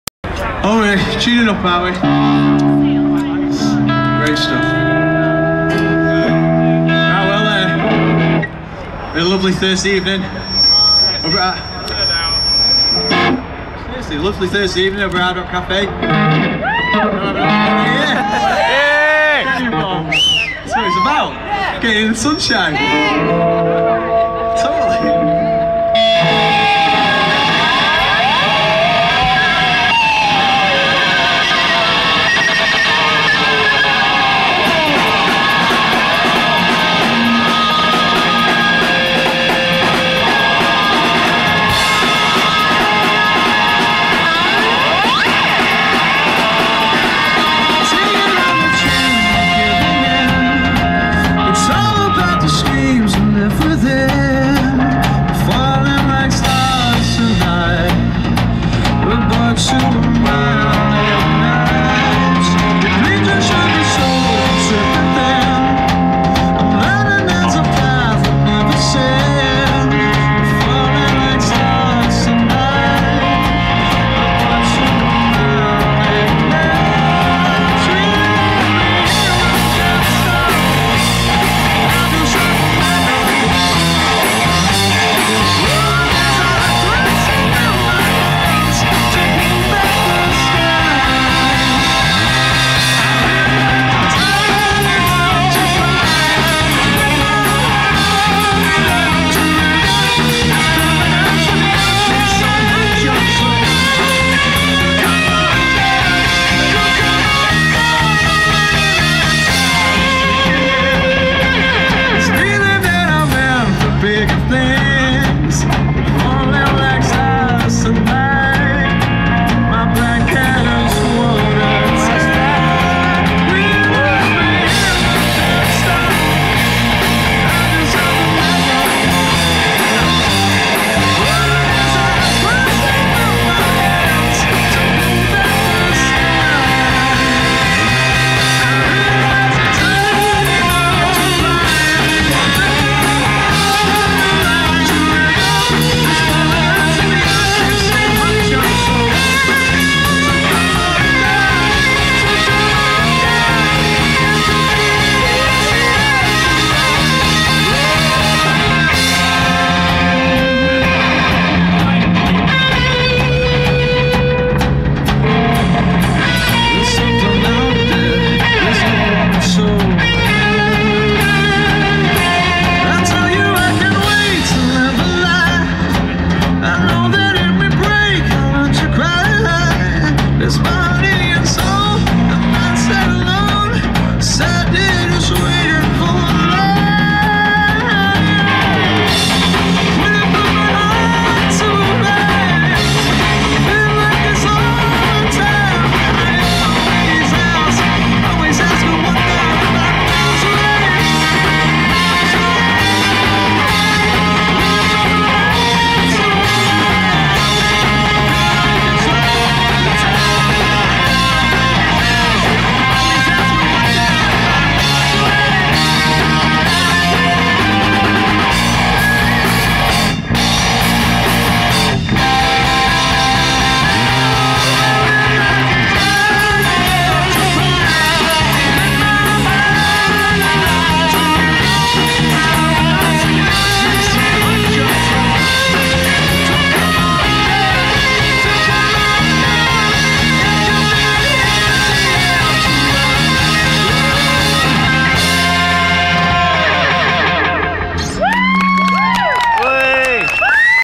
luke's gaming stolen peace at picadilly gardens
stolen peace played again in manchester and they were awsome again